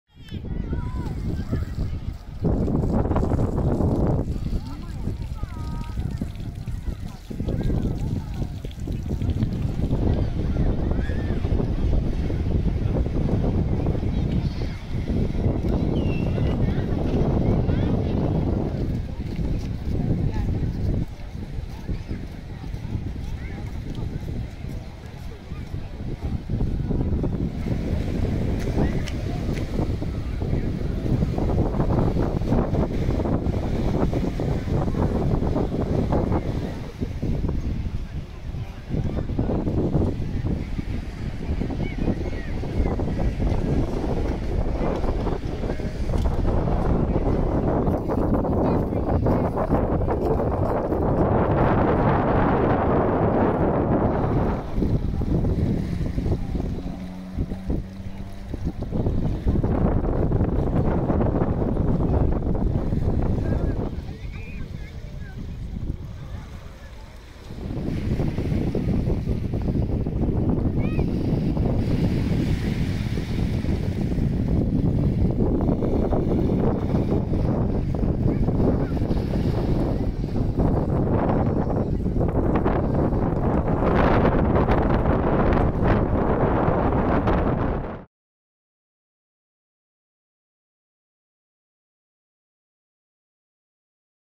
Playa de La Mata - Torrevieja (04/07/2021)
El primer domingo del mes de julio nos deja una situación meteorológica muy tranquila con temperaturas altas, elevada sensación de bochorno y viento, en general, flojo. Esta situación propicia que playas como la de La Mata presenten una gran afluencia de personas que hoy disfrutan de un mar muy tranquilo con bandera verde.